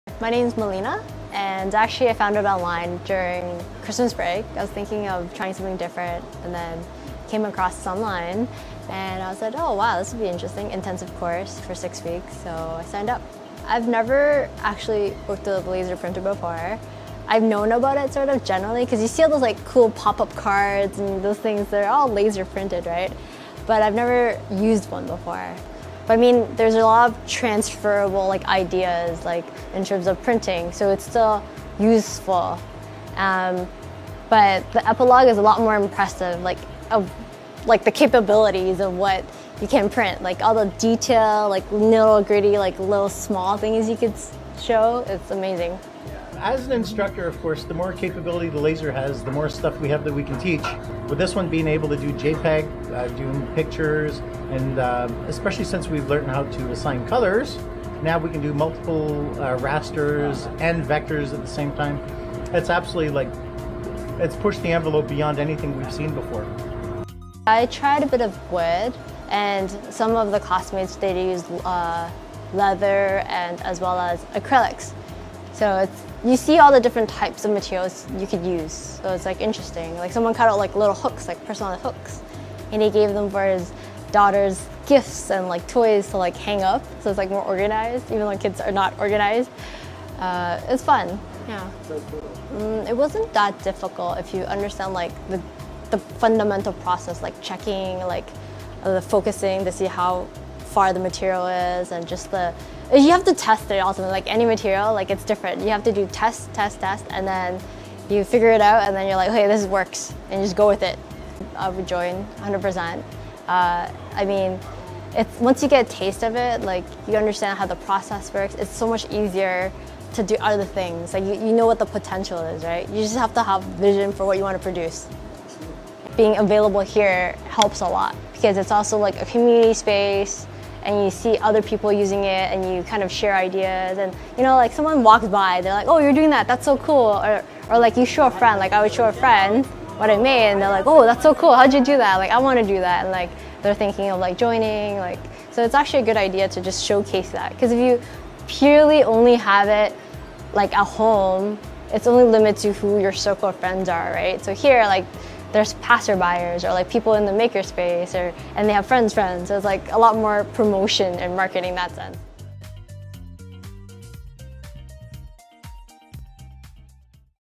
Milena - 6 Week Laser Program
Milena is a student of the 6 week intensive laser instruction program at the TTL Makerspace. She talks about learning with the Epilog Zing laser.